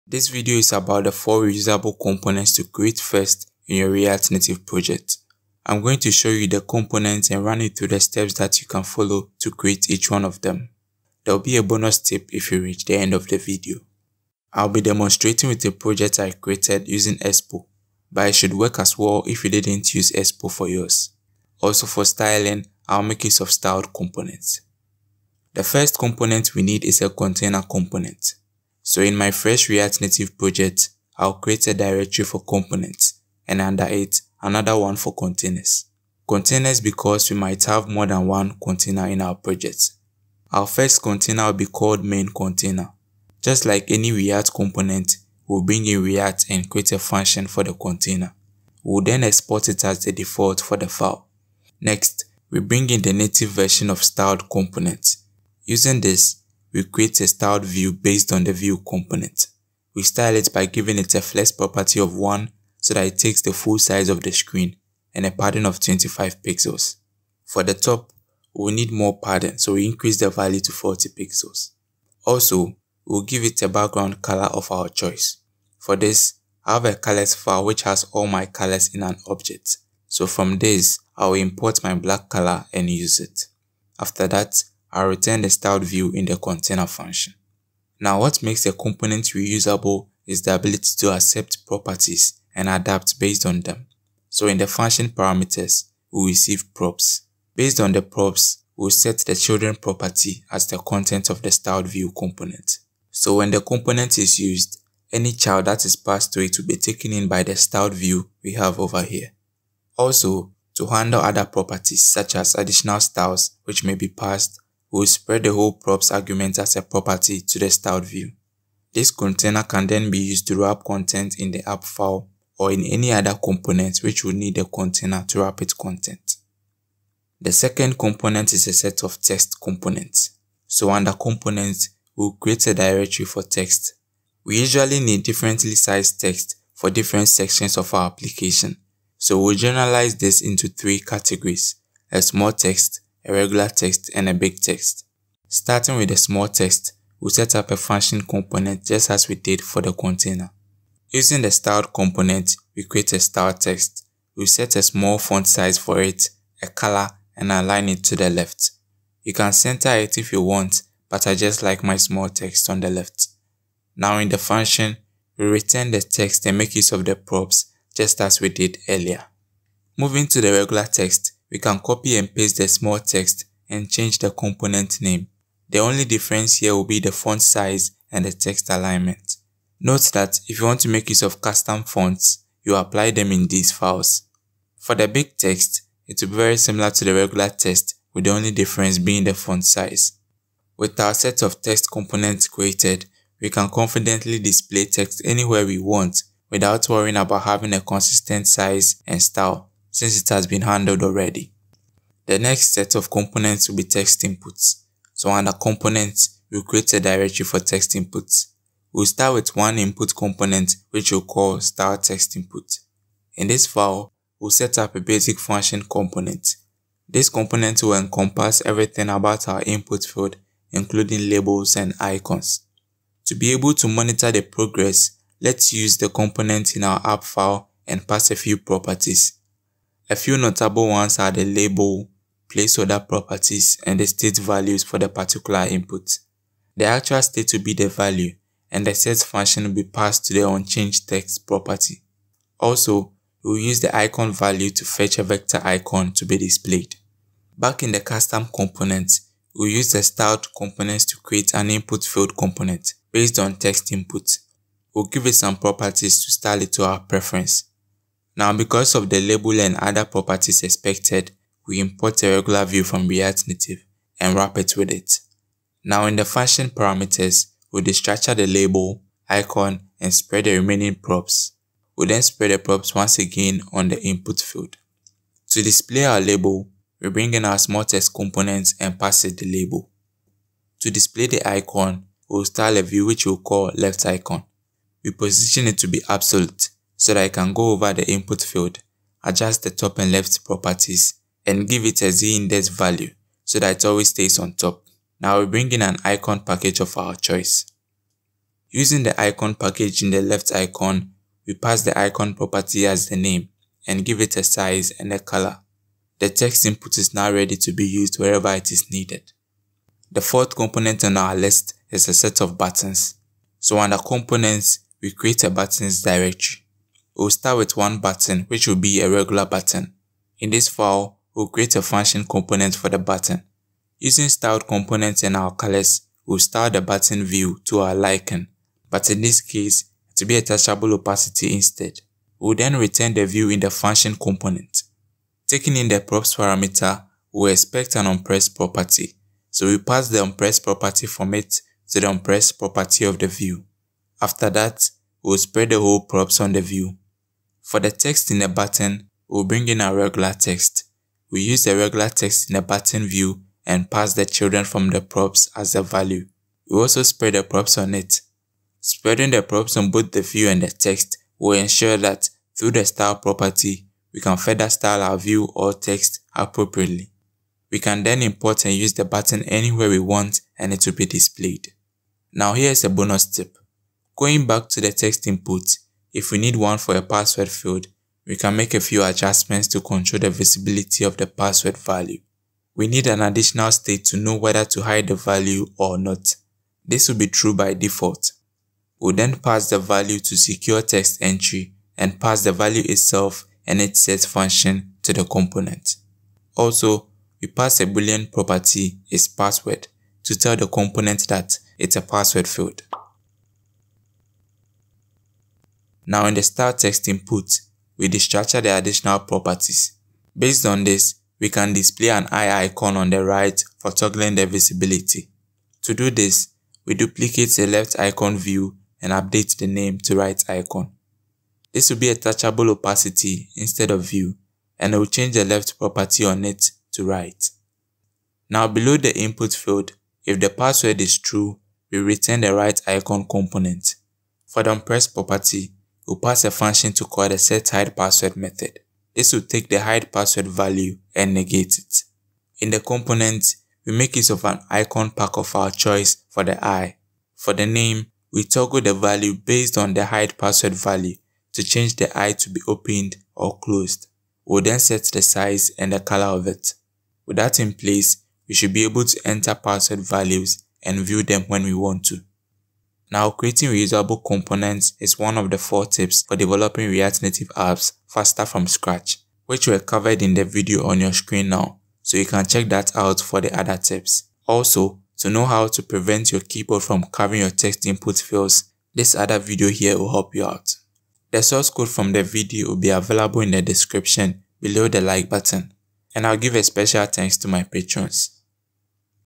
Creating The Top 4 React Native Reusable Components
In this video, we look at the top 4 reusable components to create first in your React Native Project.

LET’S WORK

TOOLS USED

CHAPTERS

00:00 Intro | What will be covered
00:24 Component #1
01:26 What makes a component reusable?
02:06 Component #2
03:21 Component #3
05:08 Component #4
06:11 Bonus Tip